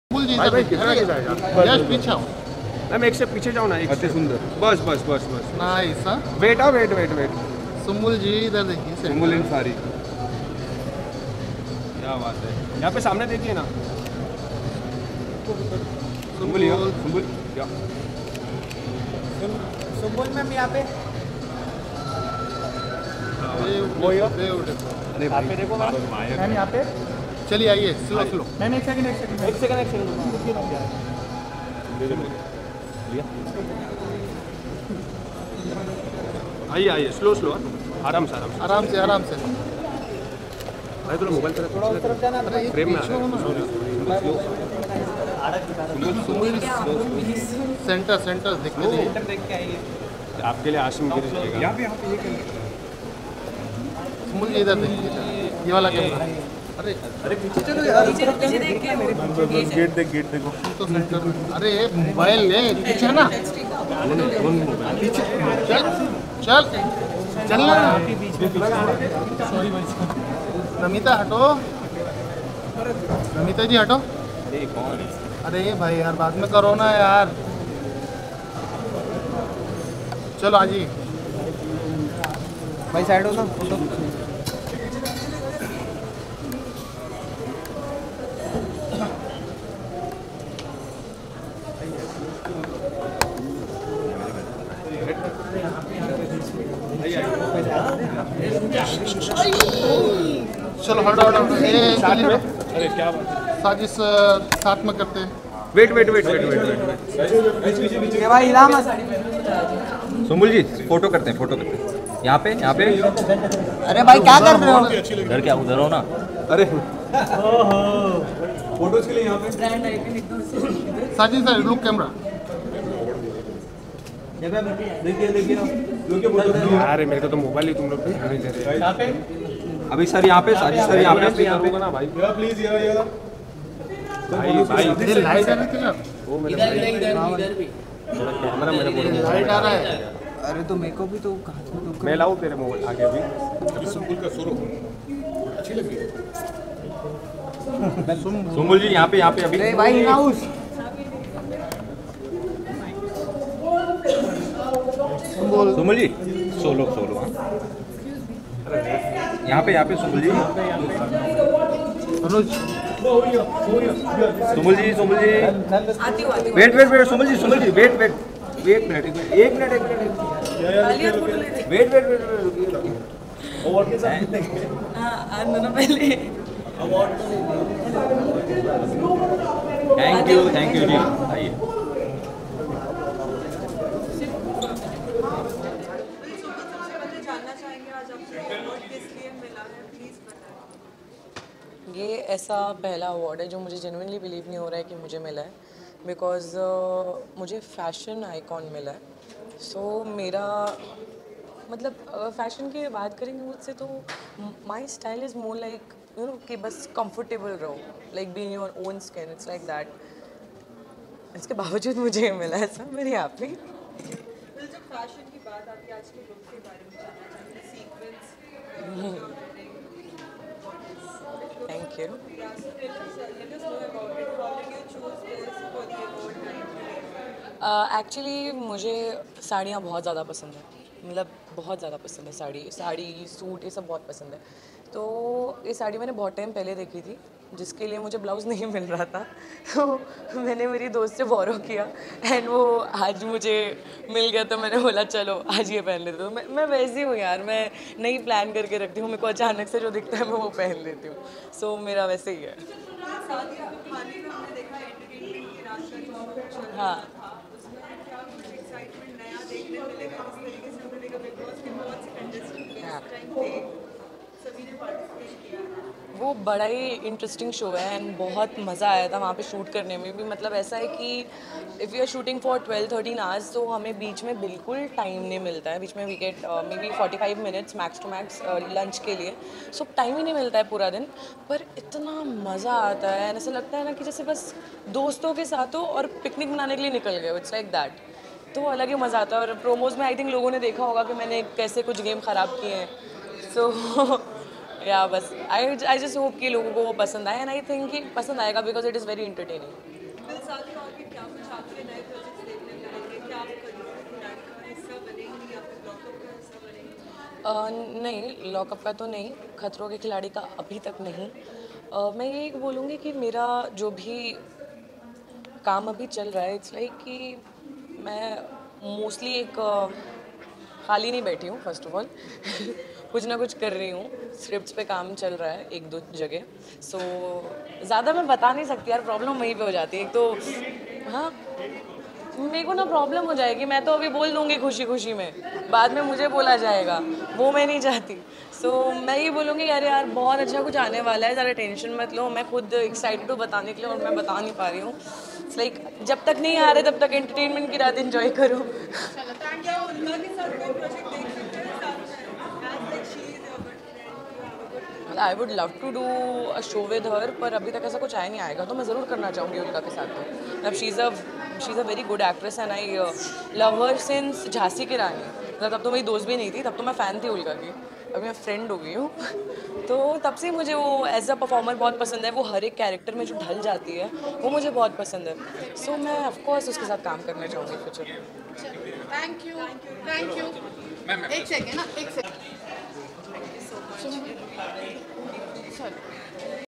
Sumbul Touqeer का Exciting interview, नीली साड़ी में दिखीं बेहद खूबसूरत, Sajid Khan से की मुलाक़ात
keywords (ignore)